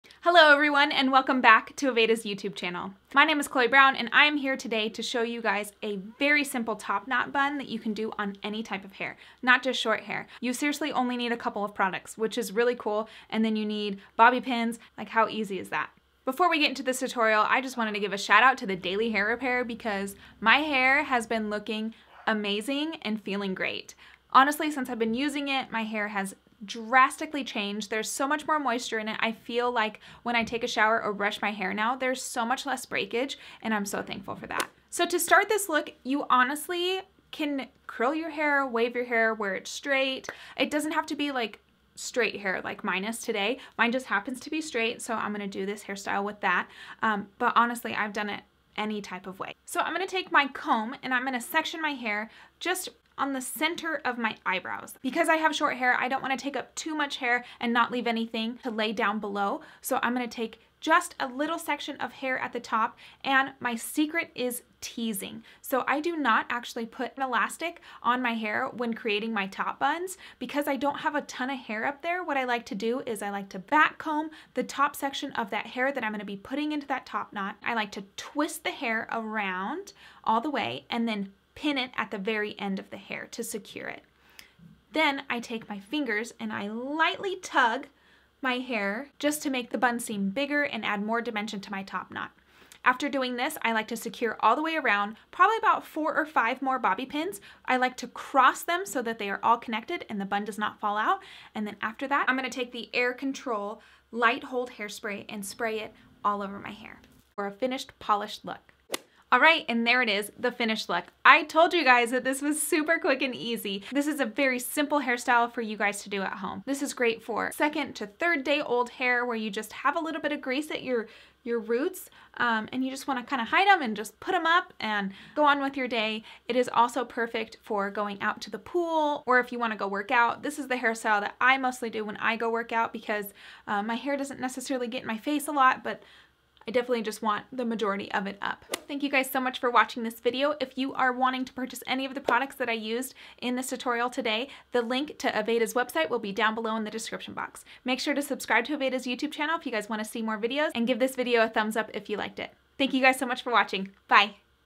Aveda How-To | Versatile Top Knot Hairstyle Tutorial with Chloe Brown
Chloe Brown shows us her favorite everyday top knot hairstyle that’s great to wear out on errands, to the gym, or anytime you want to hide excess oil in second-day hair. This style works can be done on all hair types, curly or straight, and only requires a couple of products and some bobby pins.

For this top knot style, Chloe starts out by applying Aveda Damage Remedy Daily Hair Repair from roots to ends to slightly damp hair. This leave-in treatment heals split ends, increases moisture, and helps to prevent damage from styling. Once her hair is completely dry, she uses a comb to section off the top knot by parting her hair on the center of her eyebrows, drawing two angled lines towards the back of her head. To create volume, she teases this section of hair from the back before twisting it into a small bun and securing with a bobby pin. To add even more size and volume, Chloe gently pulls at the top knot to loosen it, adjusting until it’s reached her desired shape and size. She then secures the knot all the way around with more bobby pins, making sure the pins are crossed in the middle to keep the style in place. A final security measure: Chloe spritzes the top knot with Aveda Air Control Light Hold Hair Spray, preventing any future flyaways.